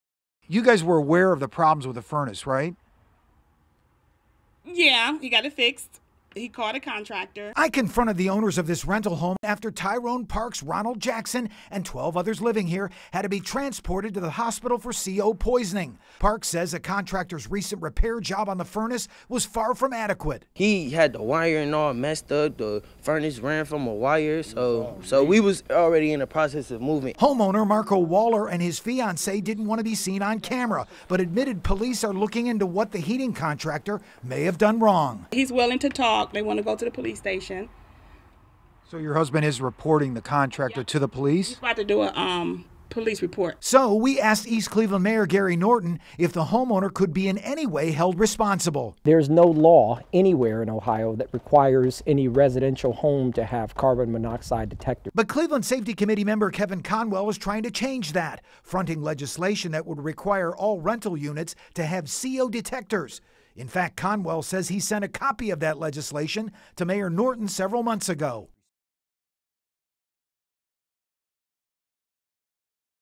14 hospitalized after E. CLE CO poisoning, should rental unit CO detectors be mandatory?
14 hospitalized after E. CLE CO poisoning, should rental unit CO detectors be mandatory?